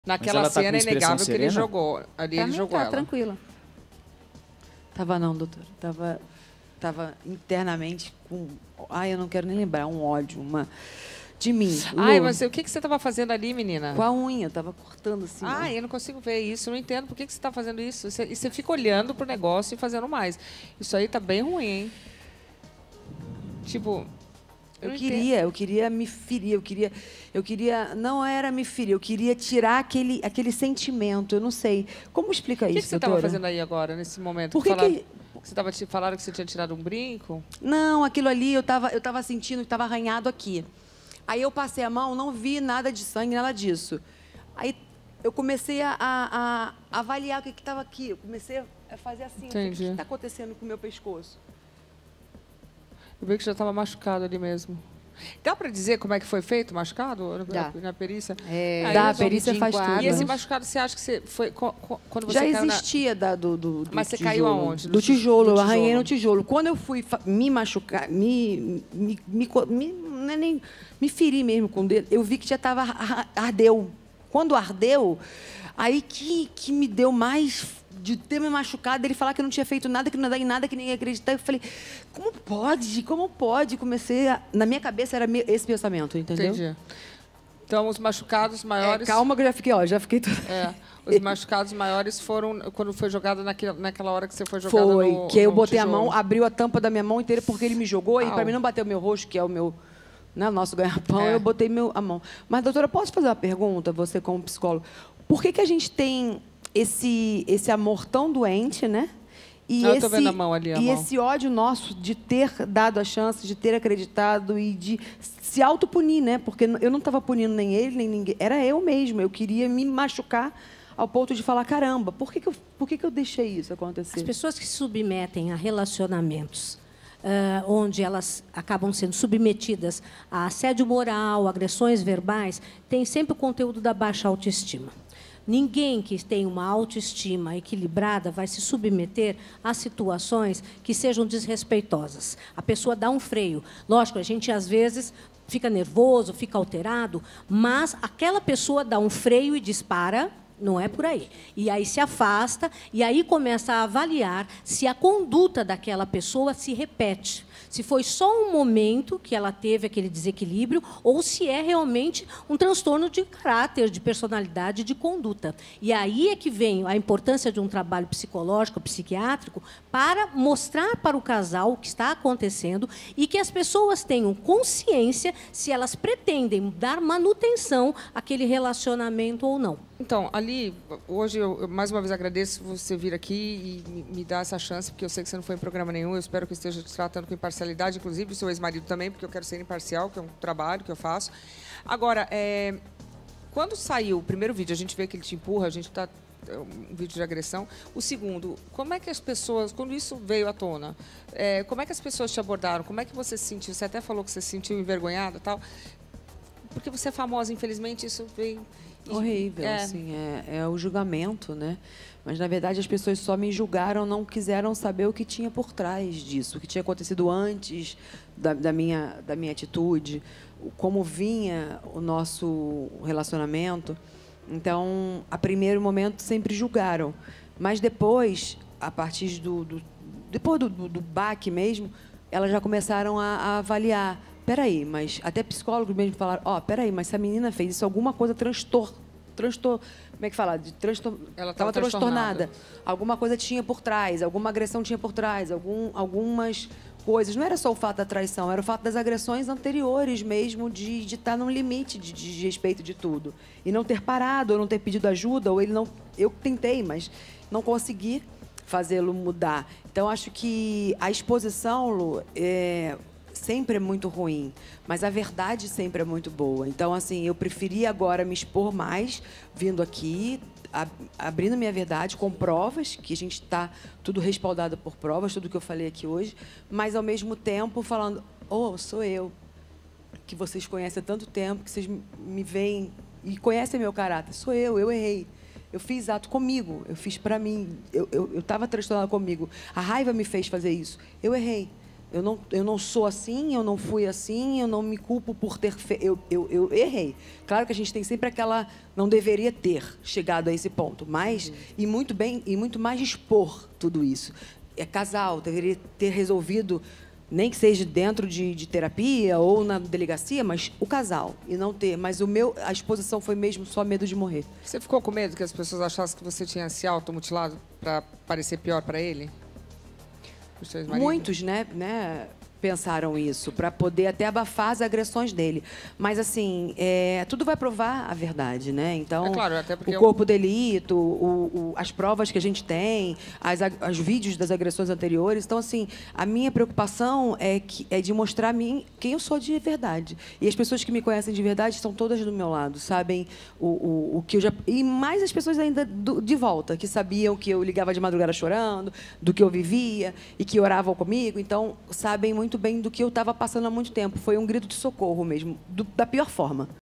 “Eu queria me ferir. Eu errei”, diz Pituxita sobre vídeo divulgado
Ana Paula "Pituxita" falou pela primeira vez sobre um vídeo divulgado nesta semana que mostra a ex-paquita se ferindo. "Eu queria me ferir. Queria tirar aquele sentimento, não sei", afirma.

Ela também negou que, como divulgado pela imprensa, os machucados não foram feitos por um objeto cortante, mas sim por arranhões de sua própria unha. Questionada sobre como foi para ela a repercussão da gravação divulgada, Pituxita explica: "As pessoas só me julgaram e não quiseram entender o que tinha acontecido antes da minha atitude (...) Eu errei, eu fiz isso comigo. A raiva me fez fazer isso, mas eu não sou assim".